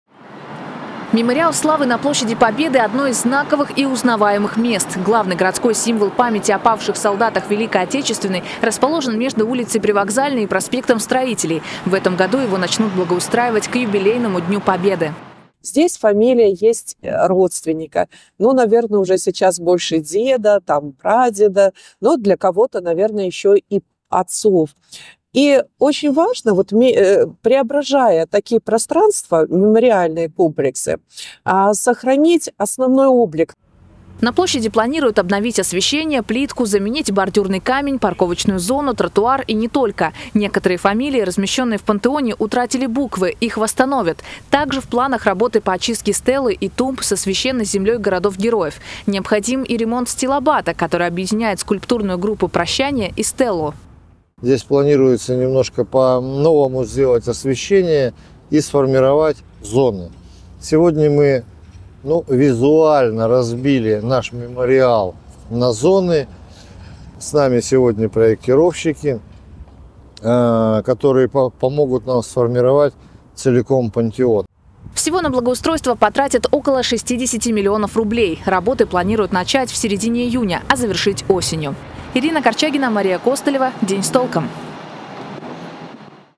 Площадь Победы в Барнауле ждет благоустройство
Мемориалу Славы в Барнауле – подарят новую жизнь. Монумент планируют отремонтировать к празднованию 80-летия Победы в Великой Отечественной войне. Об этом стало известно во время выездного совещания, которое провел глава города Вячеслав Франк. Какие еще работы по благоустройству территории планируют выполнить - смотрите в сюжете.